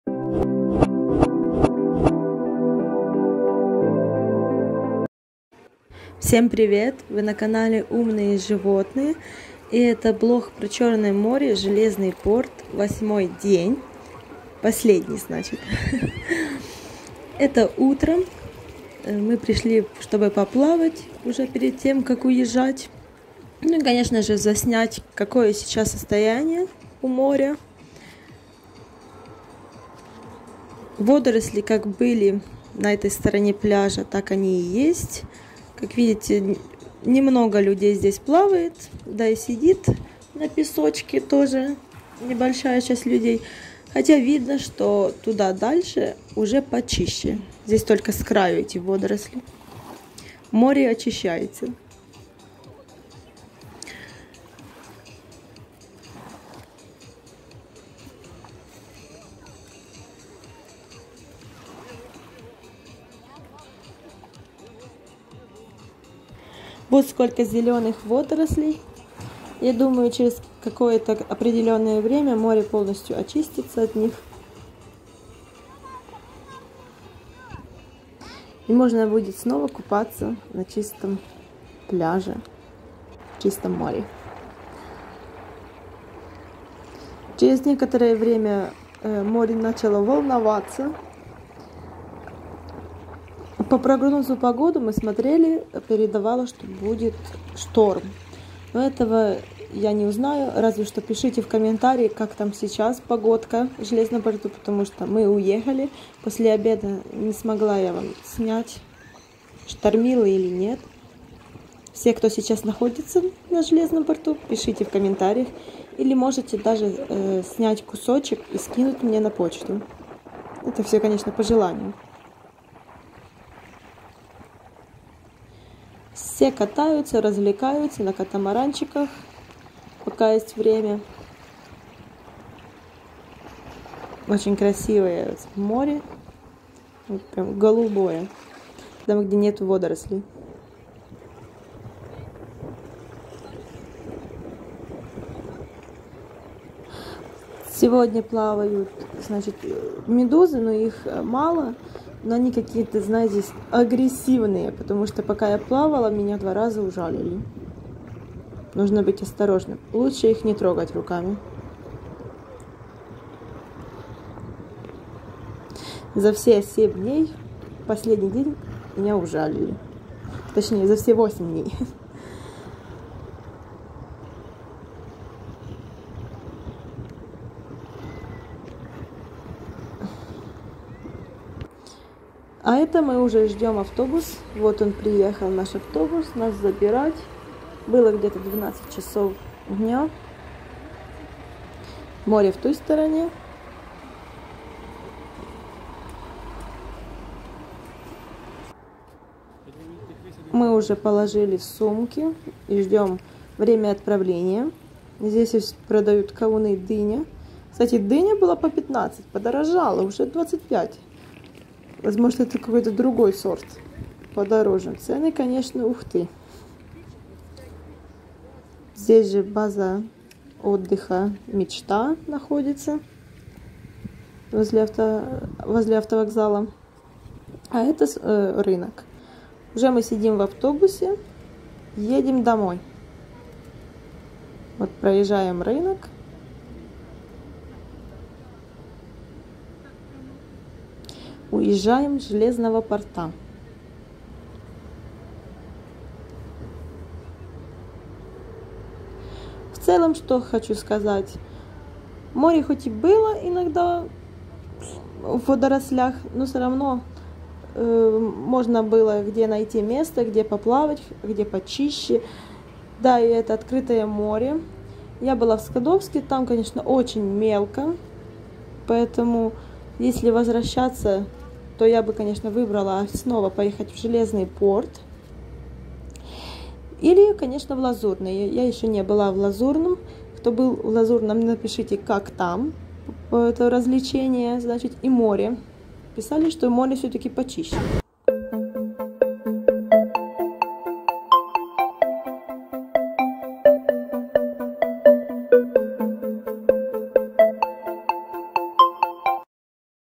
ЖЕЛЕЗНЫЙ ПОРТ 2021. ЕДИМ ДОМОЙ . БЛОГ 8/8 день, будем скучать ❤
Последний день в Железном порту , едим домой.
Пишите в комментариях как там море , видео моря можете скидывать на почту .

Мы будем благодарны Вам за поддержку на развитие
нашего канала :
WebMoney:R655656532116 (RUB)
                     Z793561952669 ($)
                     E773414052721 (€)